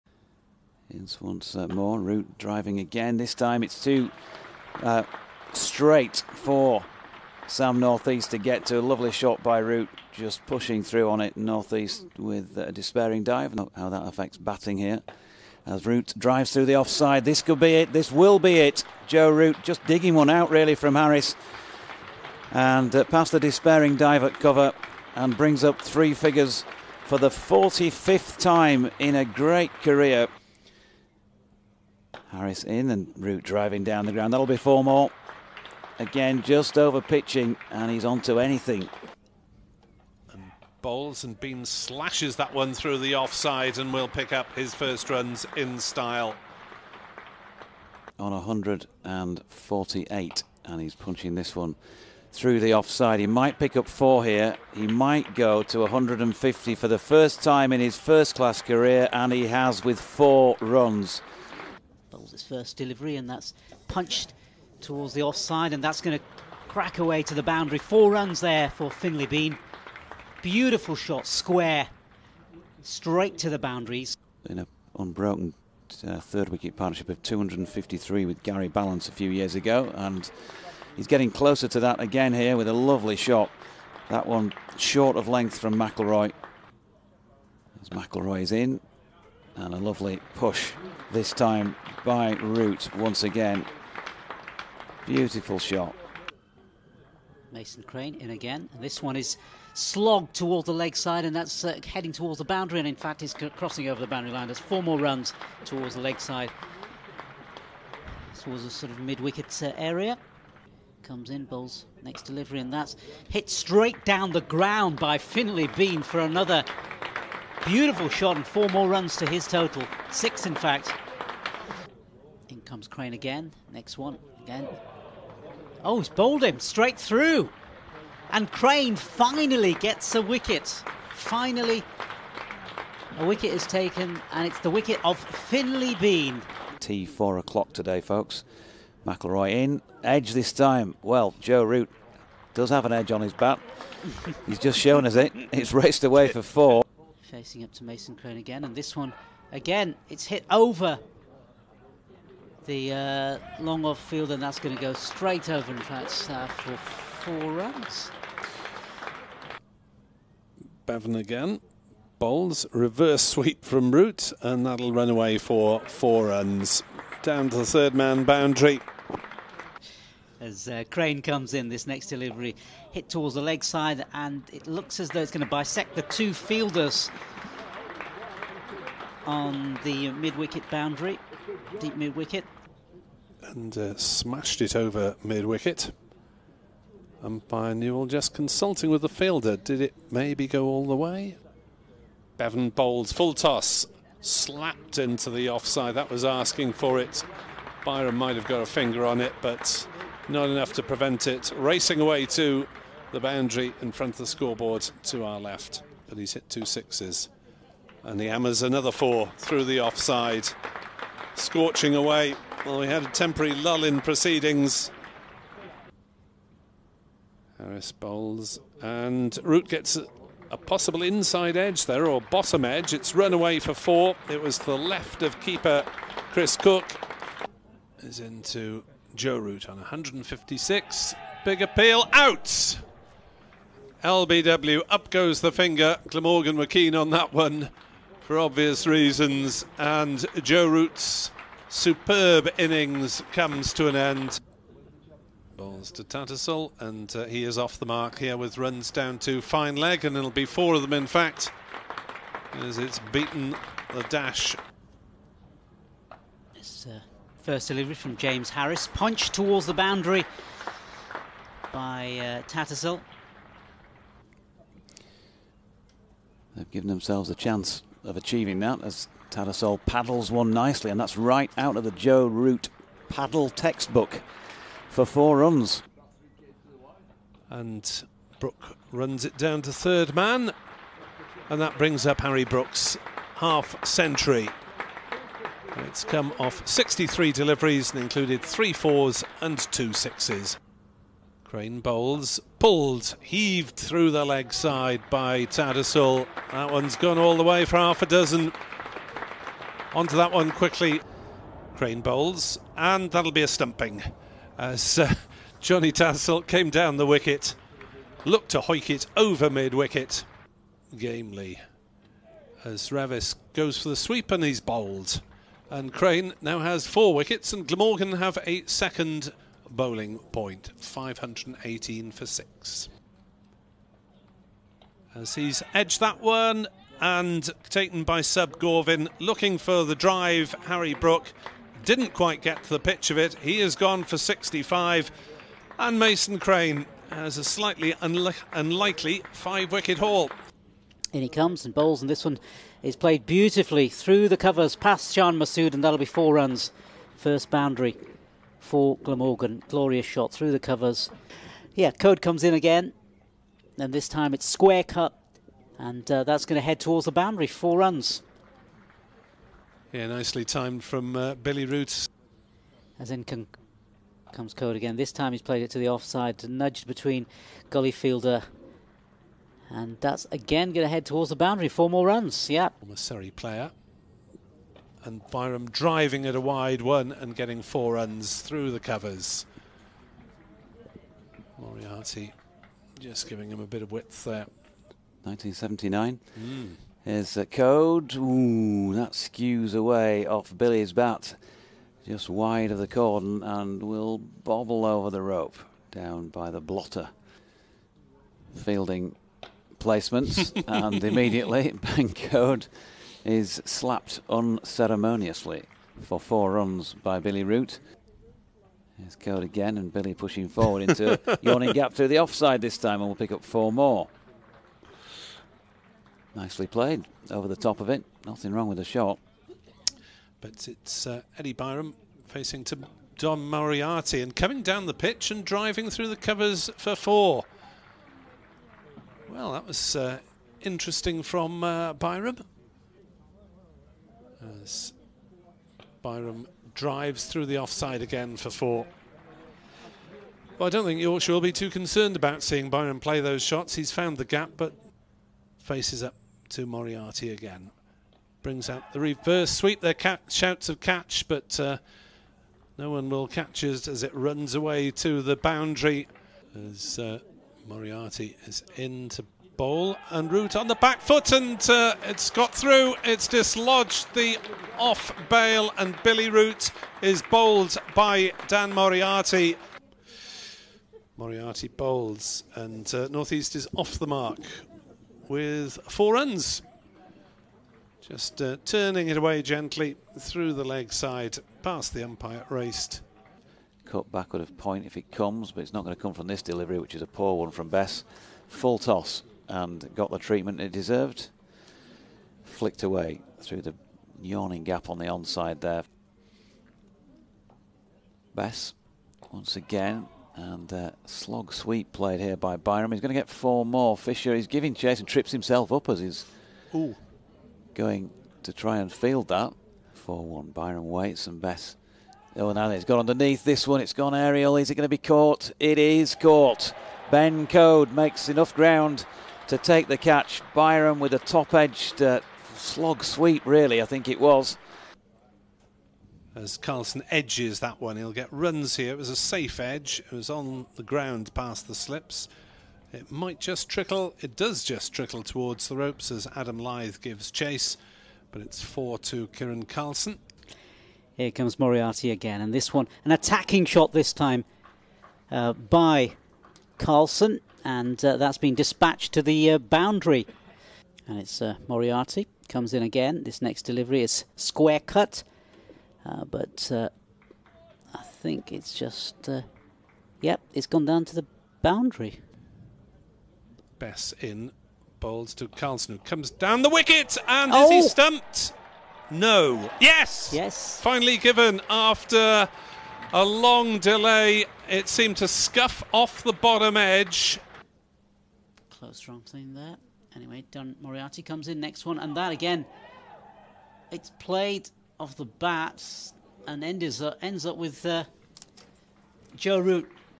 Highlights: Yorkshire vs Glamorgan - Day Three | Joe Root gets his century as Yorkshire run riot
Yorkshire declare on 519/7 with a lead of 298. Joe Root (156) gets his century and Finlay Bean (173) adds to his superb total from the previous day. Harry Brook and Jonny Tattersall with half centuries adding to the Yorkshire total.